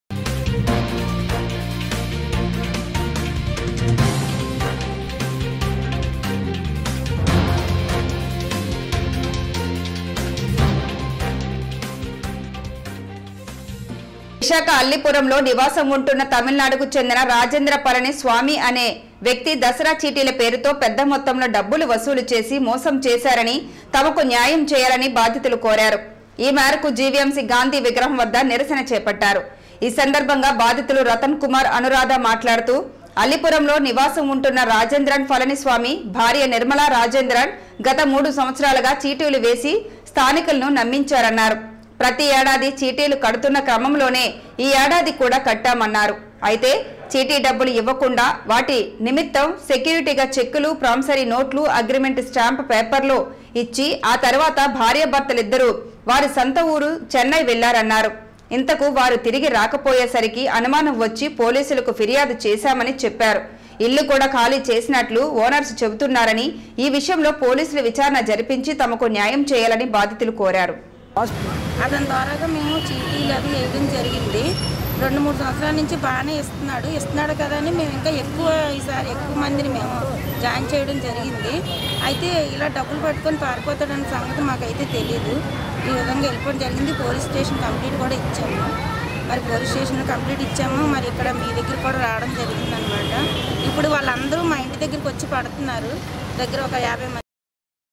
విశాఖ అల్లిపురంలో చీటీల మోసం || MY TV NEWS
విశాఖ అల్లిపురంలో నివాసం ఉంటున్న తమిళనాడుకు చెందిన రాజేంద్రన్ పళని స్వామి అనే వ్యక్తి దసరా చీటీల పేరుతో పెద్ద మొత్తంలో డబ్బులు వసూలు చేసి మోసం చేశారని,తమకు న్యాయం చేయాలని బాధితులు కోరారు.ఈ మేరకు జీవీఎంసీ గాంధీ విగ్రహం వద్ద నిరసన చేపట్టారు.ఈ సందర్భంగా బాధితులు రతన్ కుమార్ అనురాధ మాట్లాడుతూ అల్లిపురంలో నివాసం ఉంటున్న రాజేంద్రన్ పళని స్వామి,భార్య నిర్మల రాజేంద్రన్ గత మూడు సంవత్సరాలుగా చీటీలు వేసి స్థానికులను  నమ్మించారన్నారు.ప్రతి ఏడాది చీటీలు కడుతున్న క్రమంలోనే ఈ యేడాది కూడా కట్టామన్నారు. అయితే చీటీ డబ్బులు ఇవ్వకుండా,వాటి  నిమిత్తం సెక్యూరిటీ గా చెక్కులు ప్రామిసరీ నోట్లు అగ్రిమెంట్ స్టాంప్ పేపర్ లో ఇచ్చి ఆ తరువాత భార్యాభర్తలిద్దరూ వారి సొంత ఊరు చెన్నై  వెళ్లారన్నారు.ఇంతకూ వారు తిరిగి రాకపోయేసరికి అనుమానం వచ్చి పోలీసులకు ఫిర్యాదు చేశామని చెప్పా